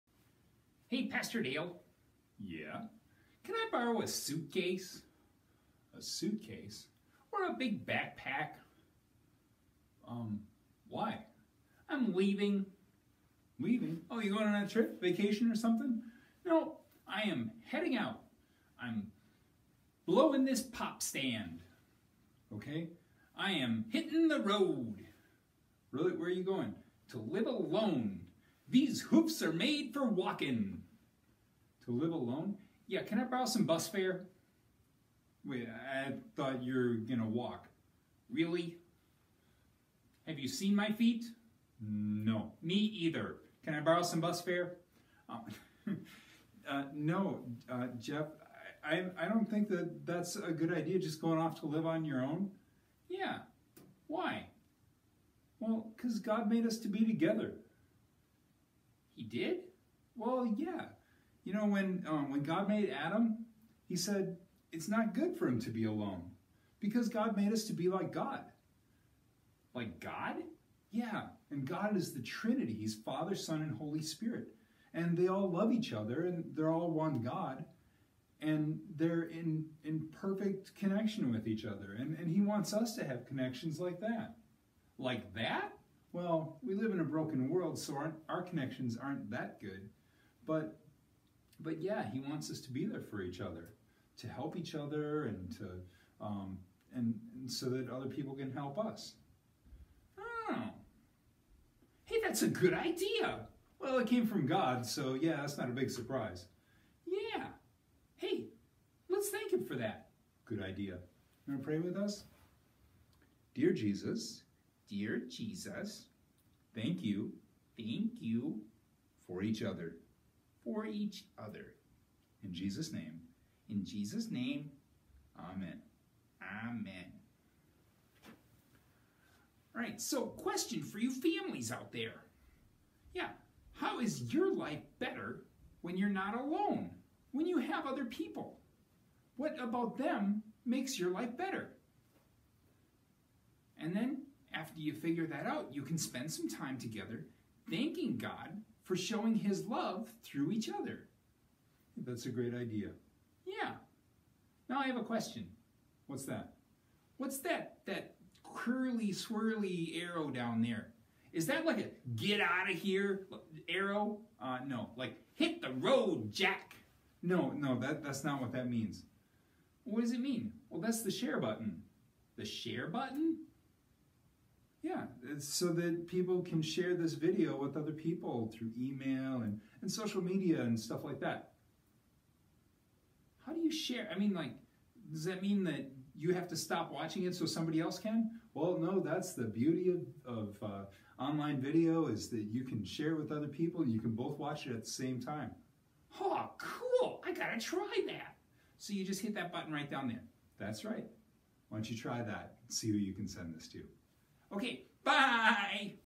Why Mutton Jeff is Ditching Pastor Dale — But Should He?
Mutton Jeff is ready to hit the road and live life on his own! 🚶🐑 But Pastor Dale isn't so sure that's the best idea. In this episode, Jeff learns why God created us to be in community and the importance of having others in our lives. Pastor Dale explains how the Trinity and our connections with others reflect God’s love and how we can be there for each other in a broken world.

💬 Join Pastor Dale and Mutton Jeff in this heartfelt yet humorous conversation about friendship, family, and faith. Can you share the love too? 🙏